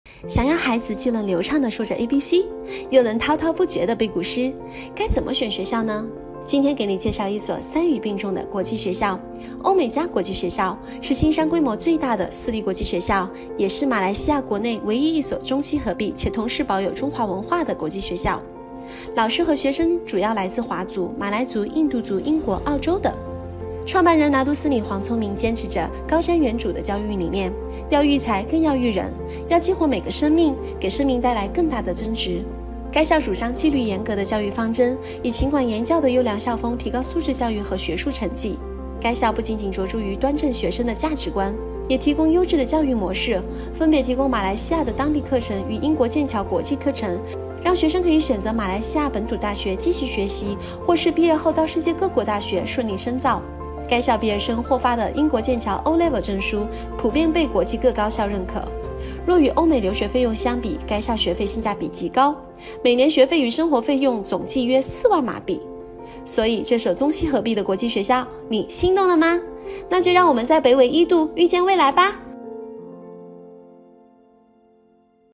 SERI OMEGA Private & International School 欧美加私立及国际学校
🏝This summer, let's visit the SERI OMEGA Private & International School with RNF Princess Cove!

📚“育”见未来 【Omega篇】
🤯想要孩子既能流畅的说着ABC，又能滔滔不绝的背古诗？
🏝这个夏天和 富力公主湾 一起去 欧美加私立及国际学校 看看！